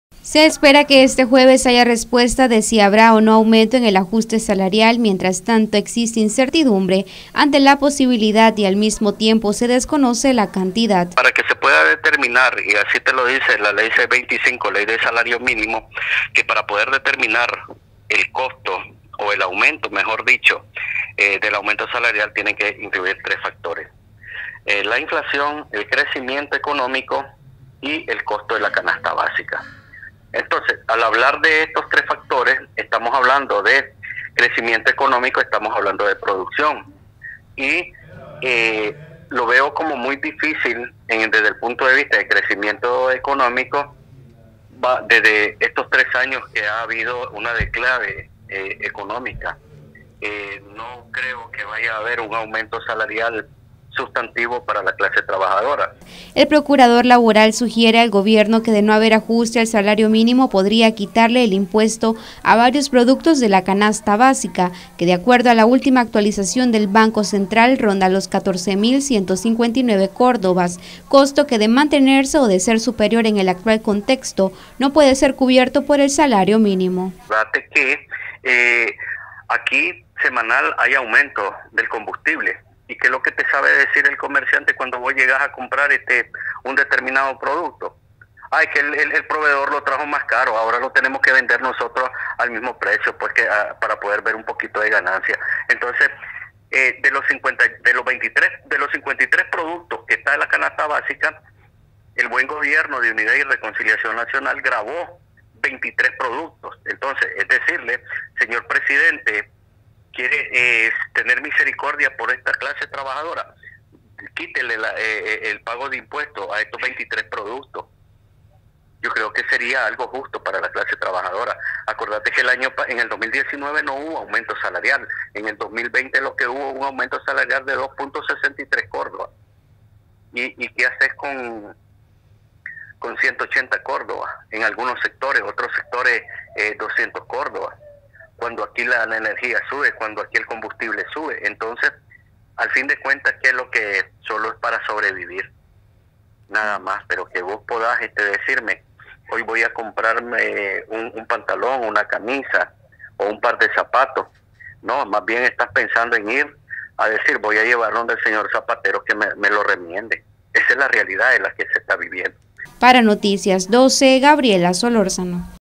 Gobierno debería quitarle el impuesto a varios productos de la canasta básica
NosResistimosAlEmbargo Hay mucha incertidumbre ante el posible aumento al salario mínimo, de no haberlo el gobierno podría quitarle el impuesto a varios productos de la canasta básica sugiere procurador laboral.